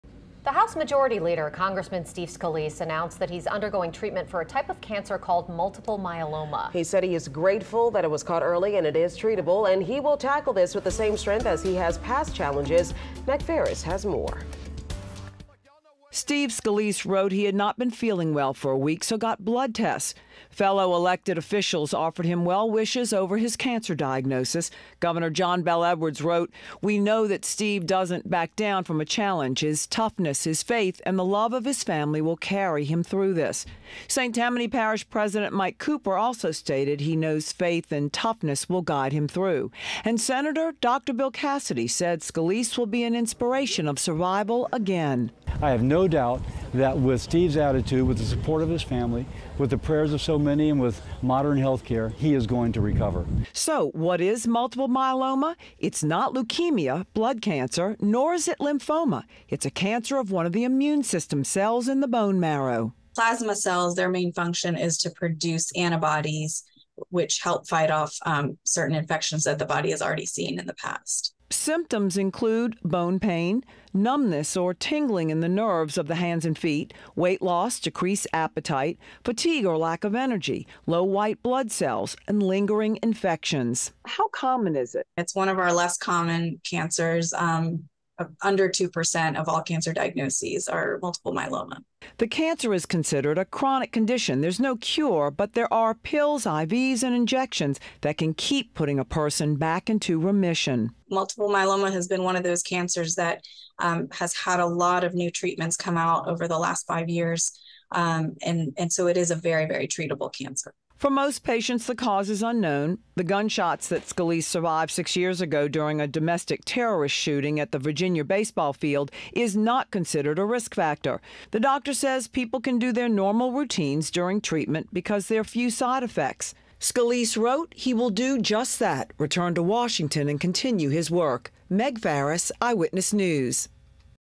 What you need to know about House Majority Leader Steve Scalise's very treatable blood cancer – Mult
House Majority Leader, Congressman Steve Scalise, announced that he is undergoing treatment for a type of cancer called multiple myeloma.